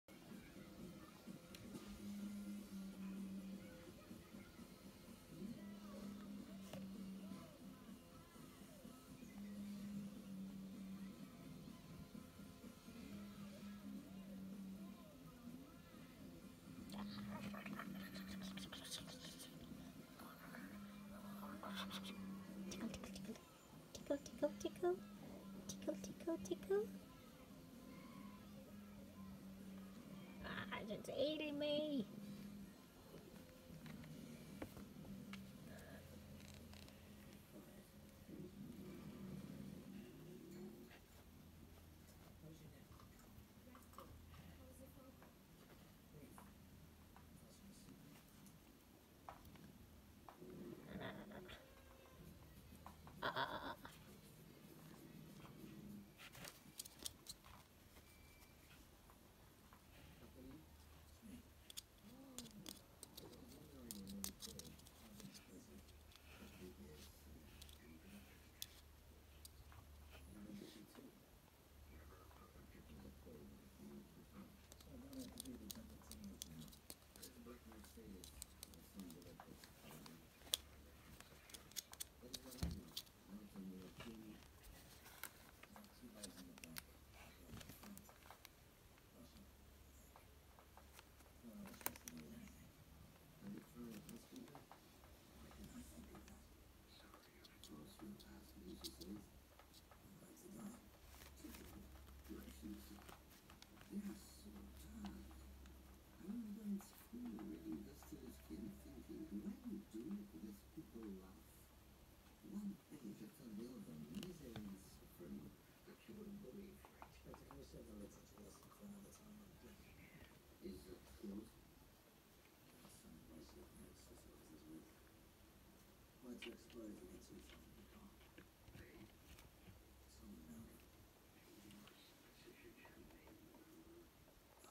Cute kitten encouraging her to play
First day Cheeta aka Comet feels safe in her cat carrier - trying to encourage her to play